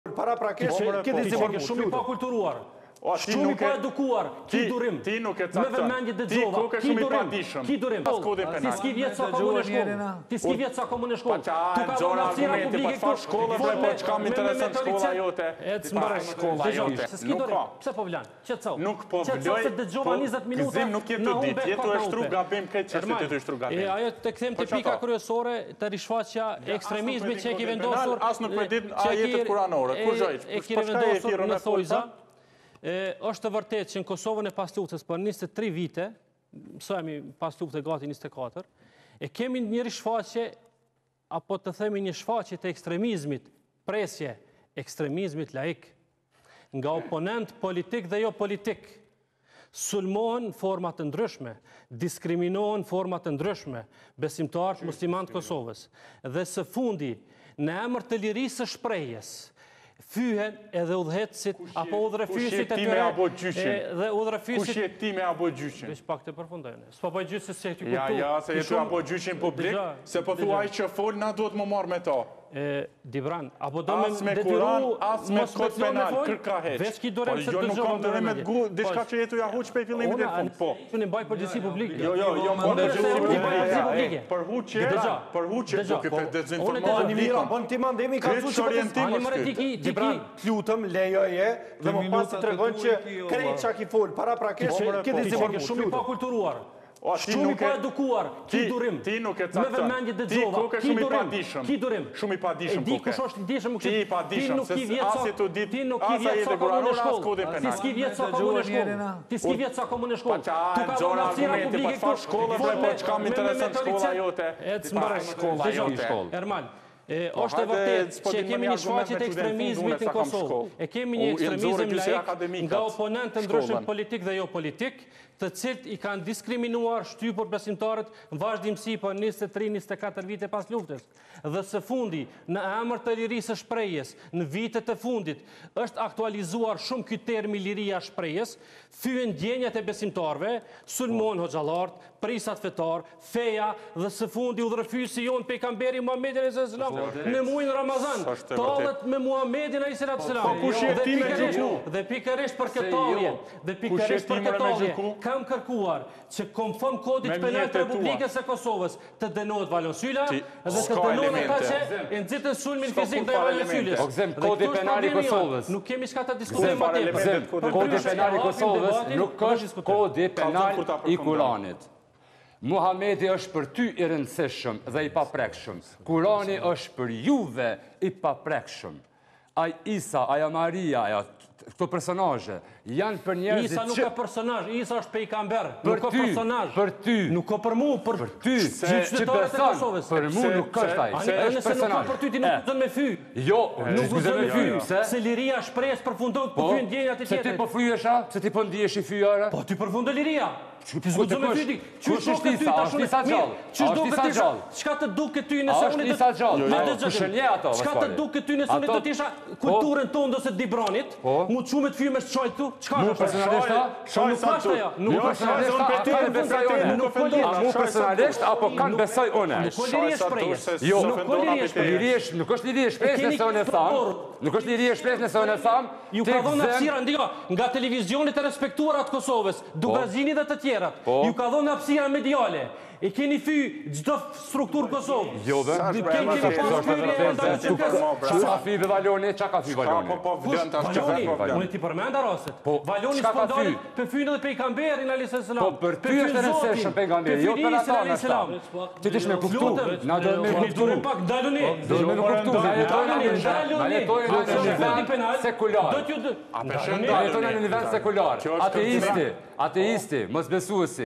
"Ti ski vjet sa kom une shkoll"/ A ju kujtohet kjo perplasje ne mes Dibran Hoxhes e Gezim Kelmendit
"Ti ski vjet sa kom une shkoll", "Qenke i padijshem" / A ju kujtohet kjo perplasje ndermjet Dibran Hoxhes dhe Gezim Kelmendit